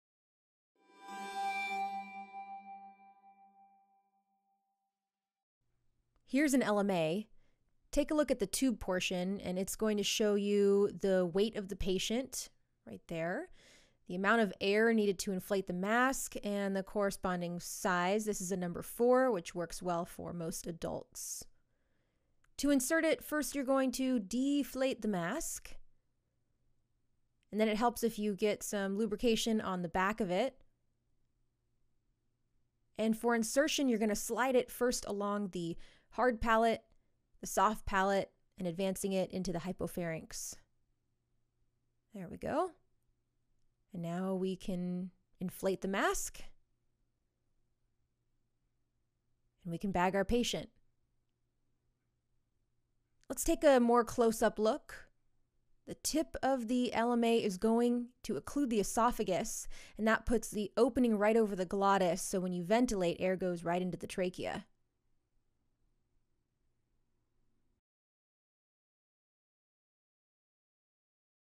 LMA Supraglottic Airway
How to place an LMA supraglottic airway, by Jess Mason, MD.

for all of your Emergency Medicine education!